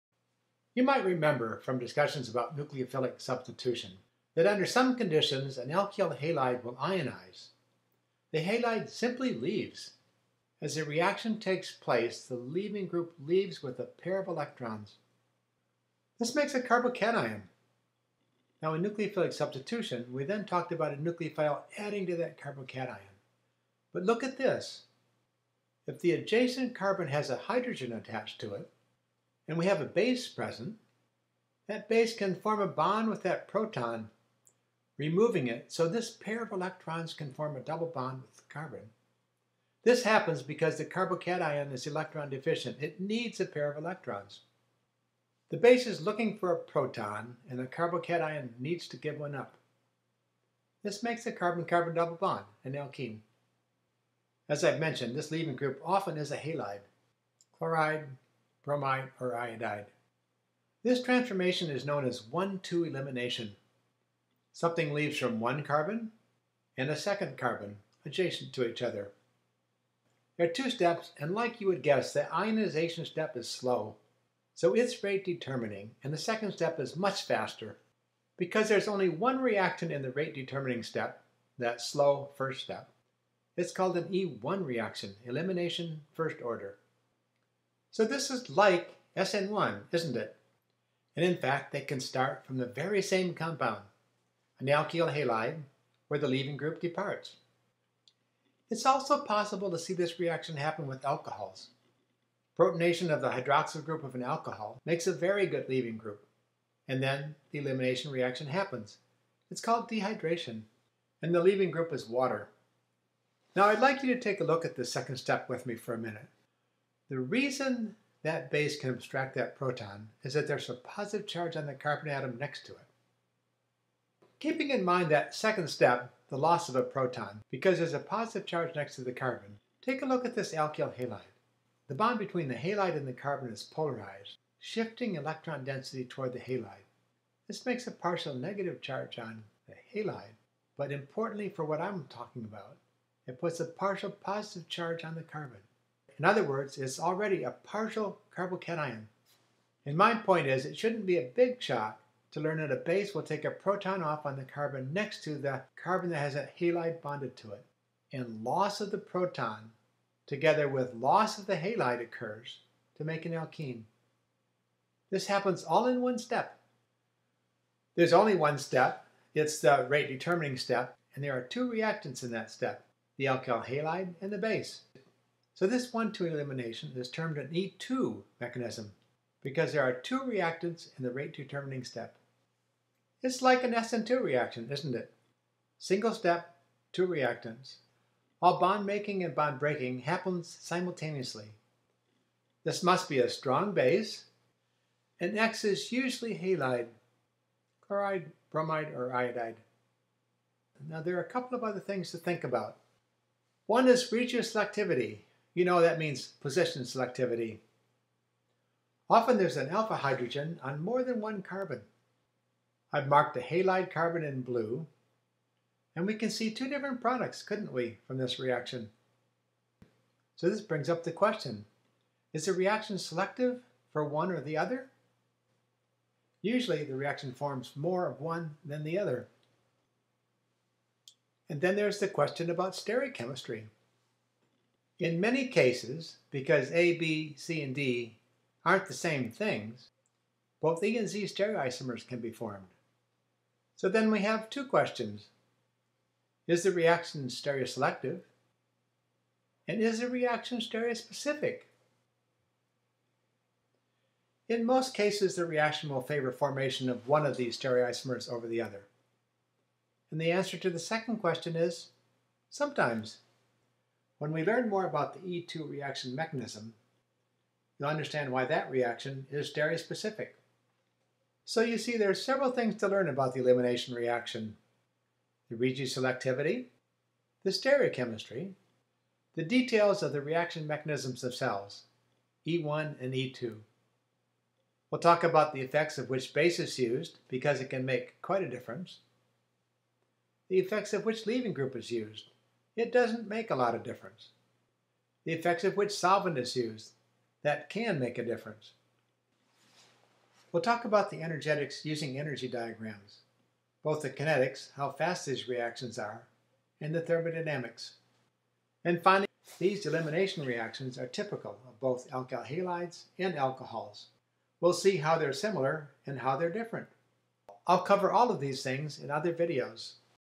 Overview of 1,2 Elimination Reactions
Describes the basic aspects of 1,2 elimination reactions, including the E1 and the E2 reaction mechanisms.